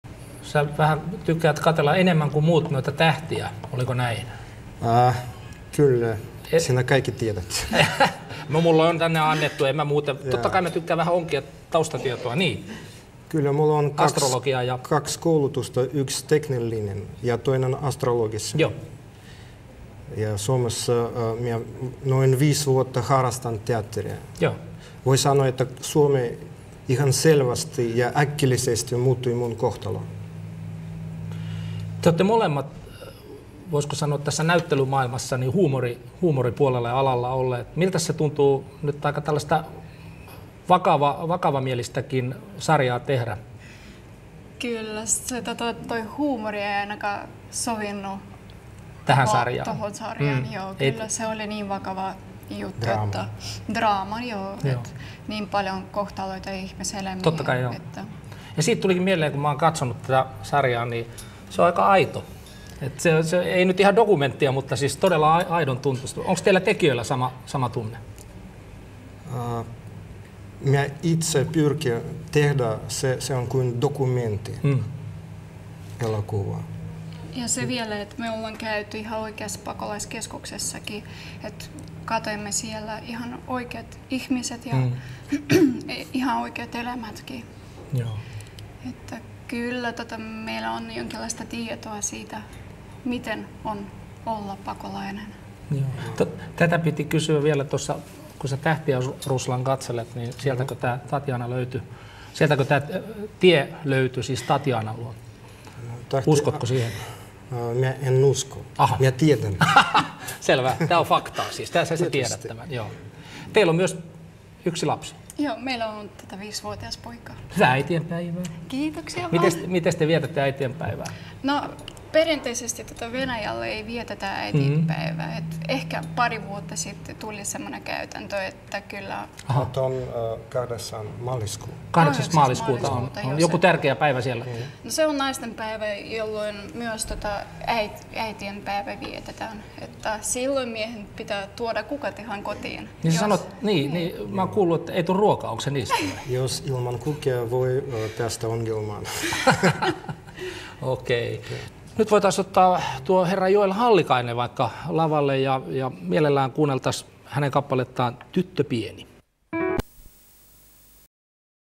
Ruslan and Tatiana Susi. Poikkeustila. 2
Interview with the actors. Tatiana Susi and Ruslan Susi